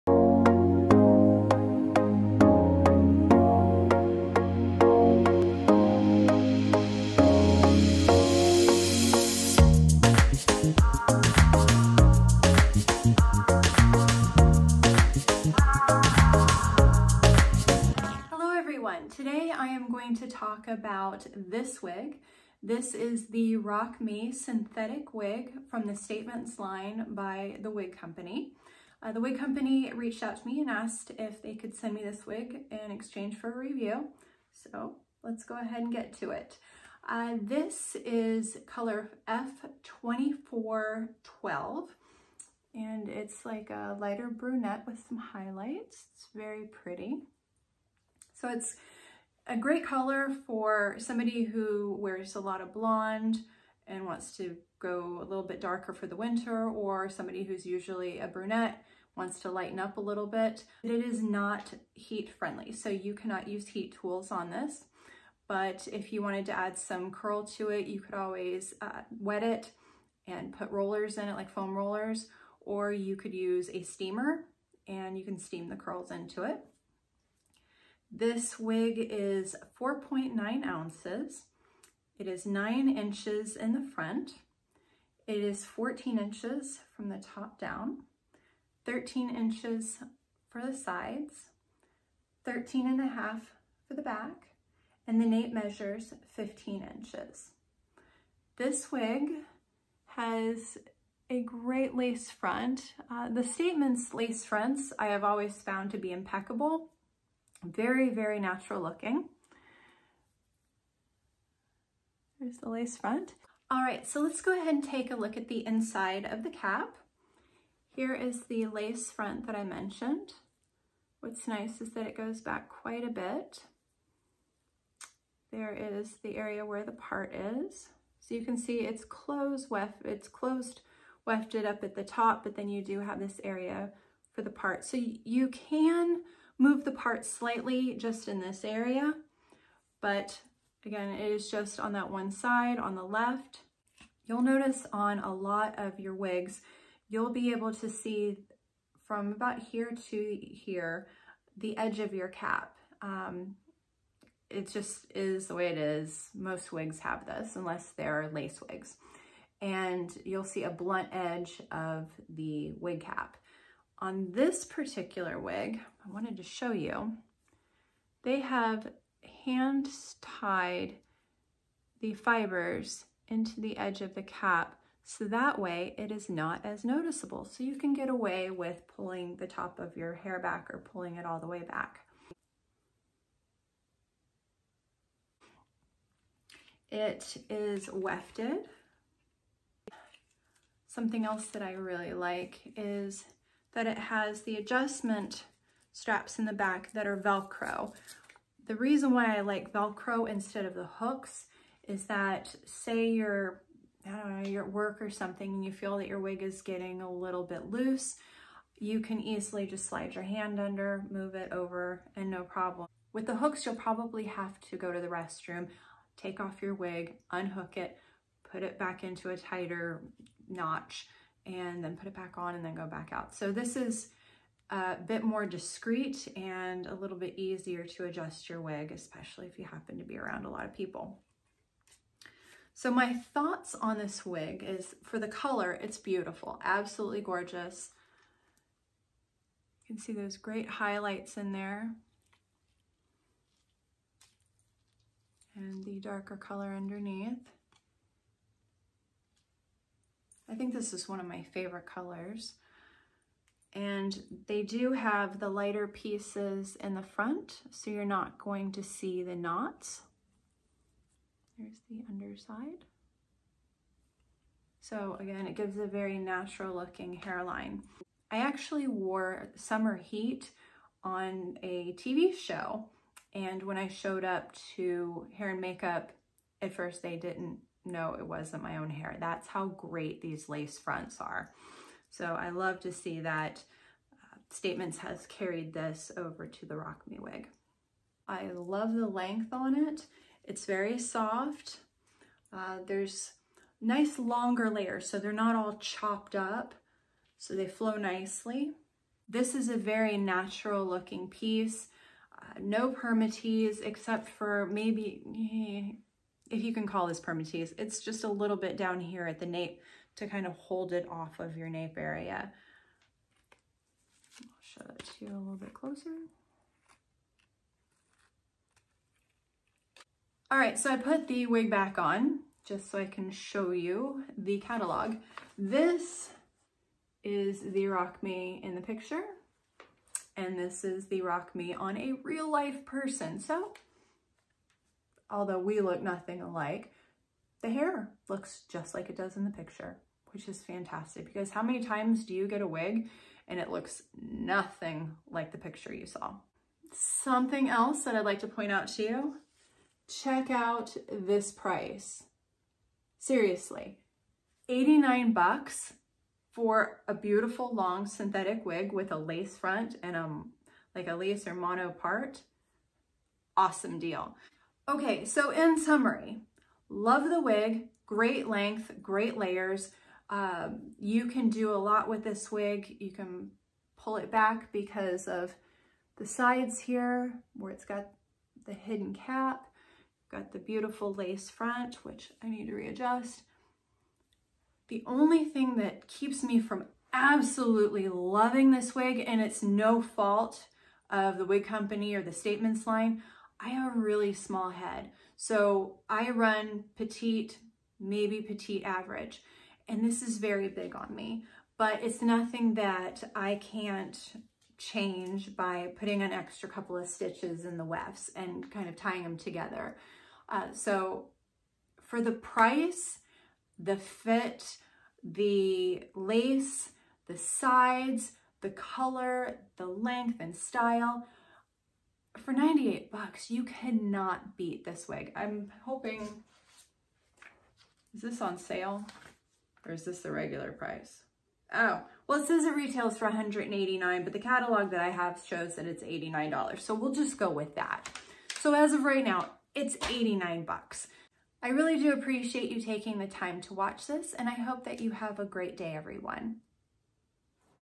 Rock Me Wig by Statements from The Wig Company color F24/12
An overview of the Rock Me wig by Statements only available at The Wig Company. This wig was gifted to me by The Wig Company in exchange for an honest review. Full disclosure, I just received the wig and have not worn it longer than it was on for the video and cannot attest to how it wears through the day. I do have two other Statements wigs that have been worn longer/multiple times that have held up well.